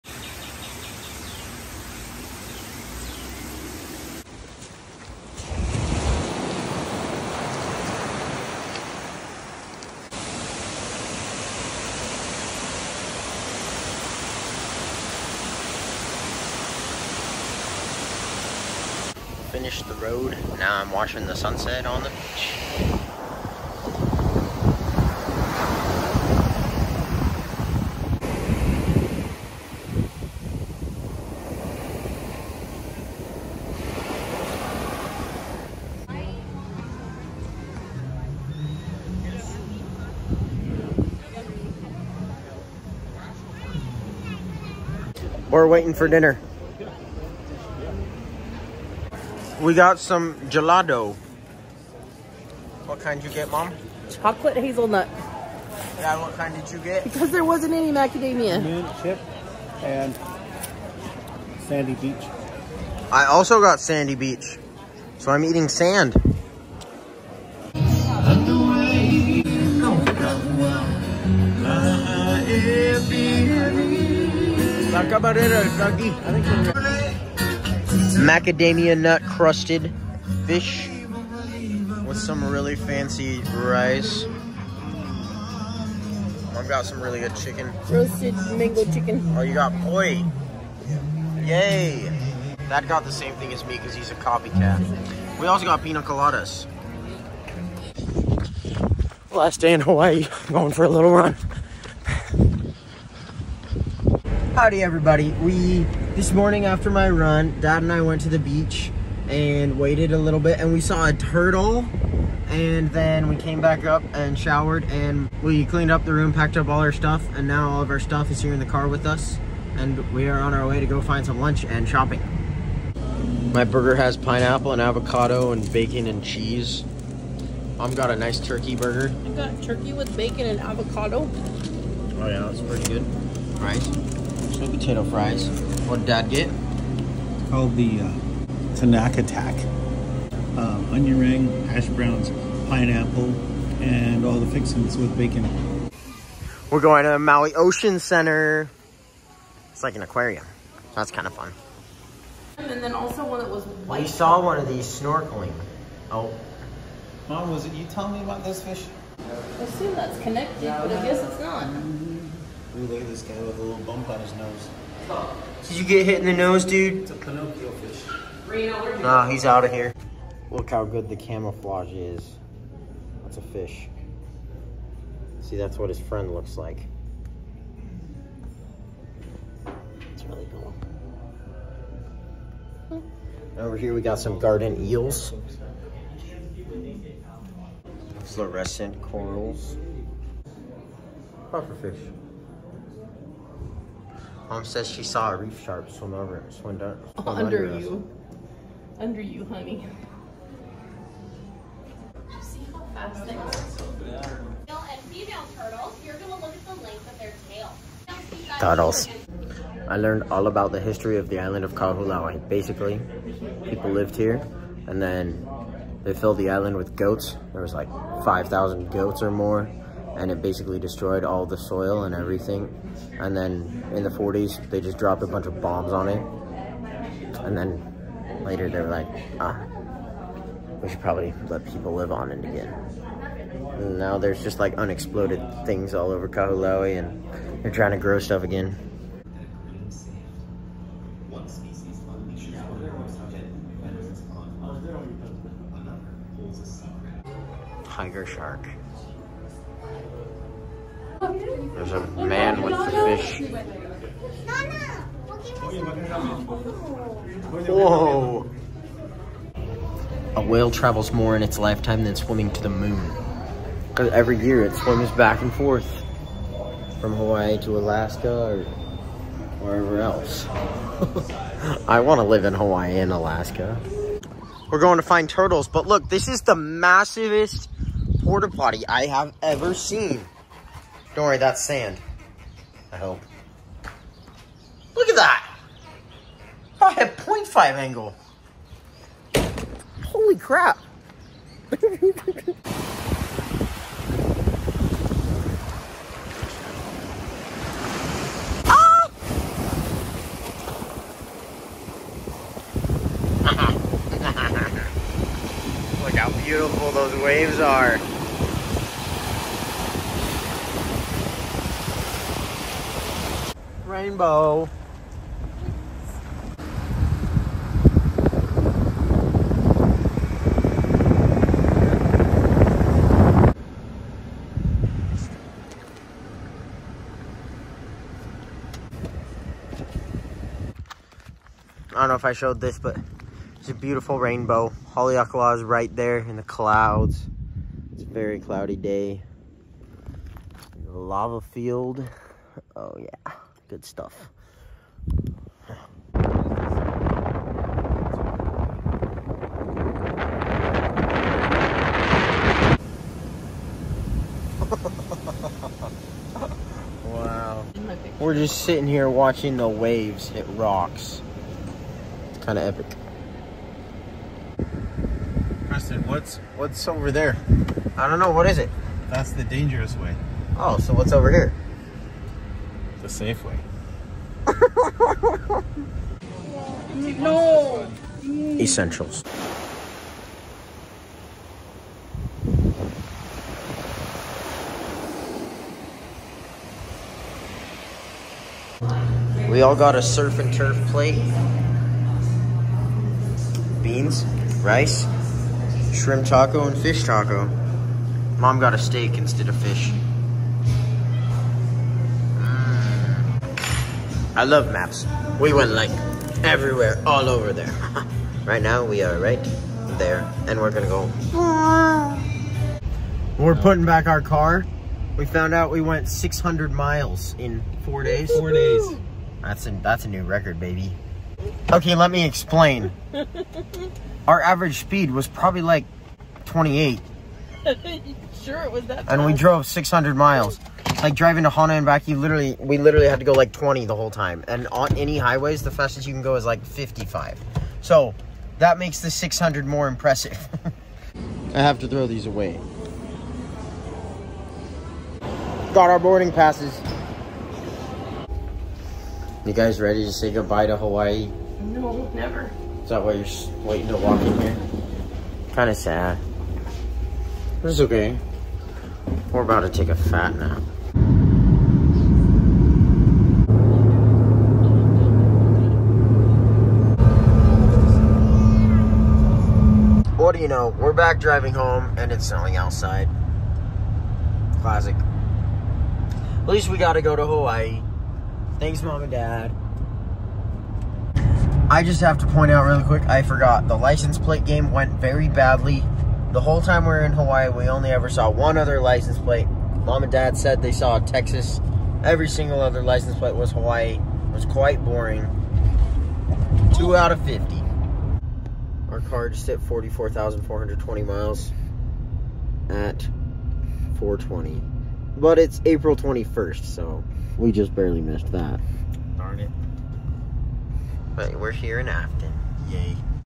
Maui Vlog (Days 4+5)
The adventures of my parents and I as we close out my senior trip to Maui with some exploring, experiencing the culture, and a visit to the Maui Ocean Center, as well as our journey home!